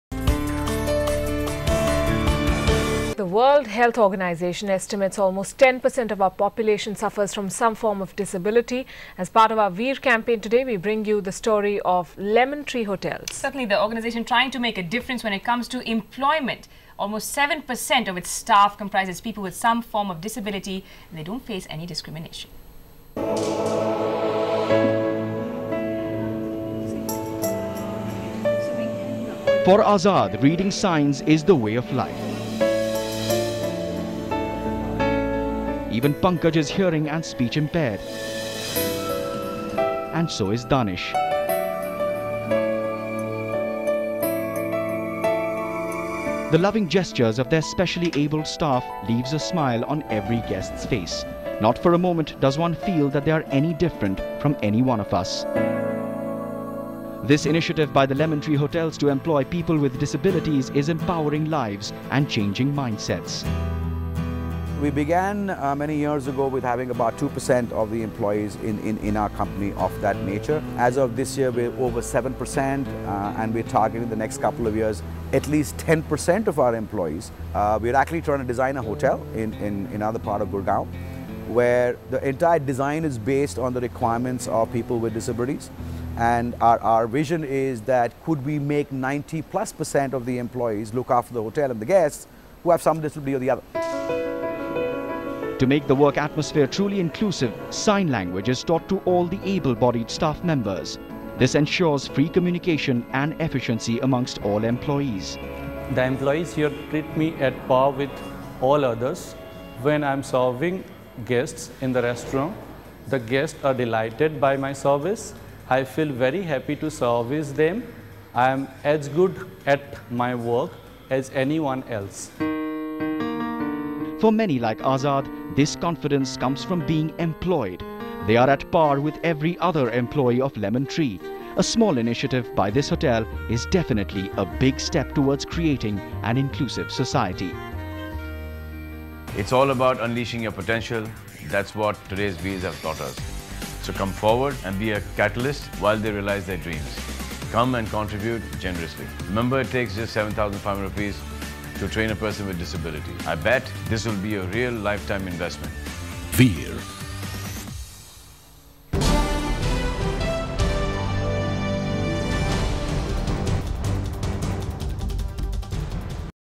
Veer Lemon Tree Hotels encourages employment of people with disabilities
According to the World Health Organization estimates, almost 10 per cent of our population suffers from some form of disability. As part of Being Human Foundation, CNN-IBN and American India Foundation's Veer campaign, we bring you the story of Lemon Tree Hotels.

The organization is trying to make a difference in employment as almost 7 per cent of its total staff comprises of people with some form of disability.